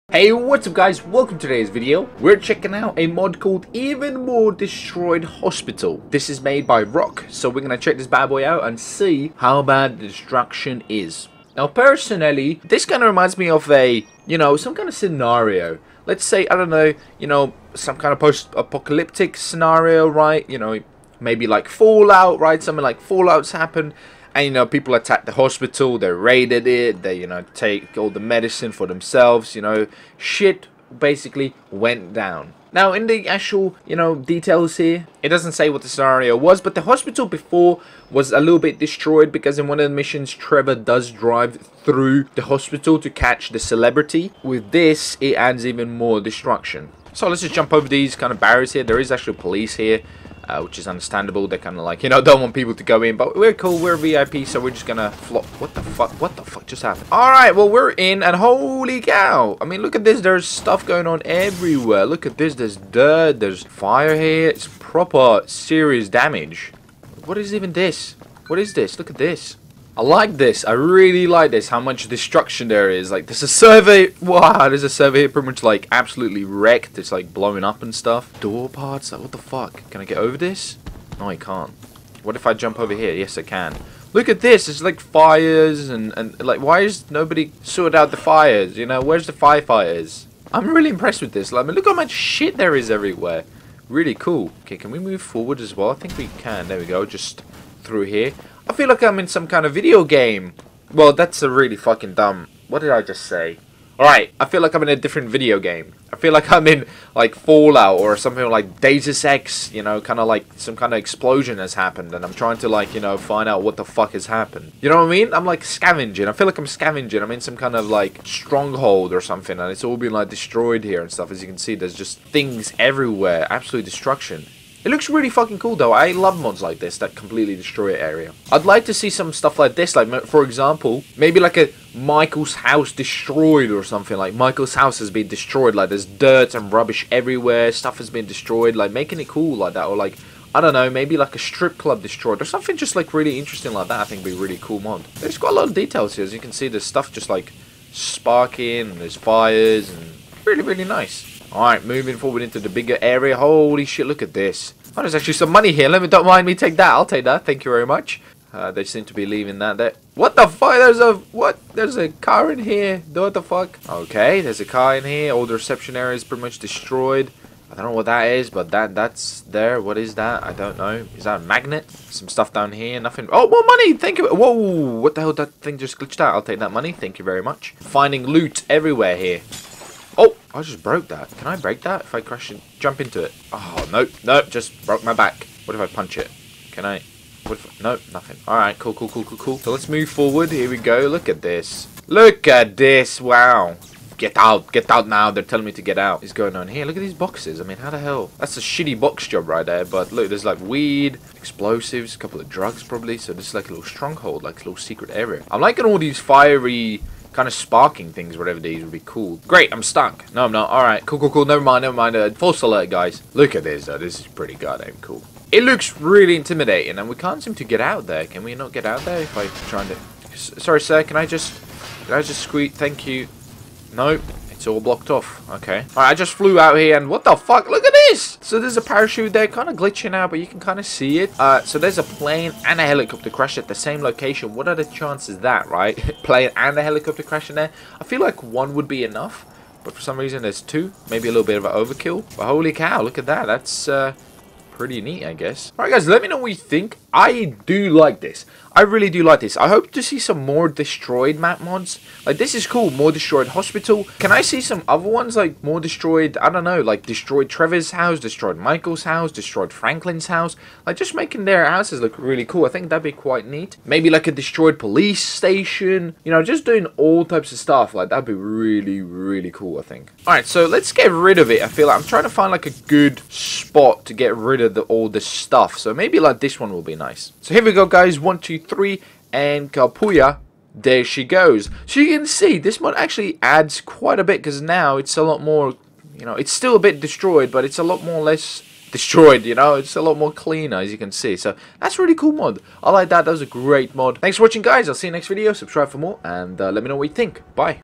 MERCENARIES ATTACKED HOSPITAL & DESTROYED IT
Follow on Twitch for more Live Streams!

I made the scenario that this hospital was attacked by some evil mercenaries. SO thats why this hospital is all messed up and looks terrible. Let me know what you think guys I really like the distraction in this mod.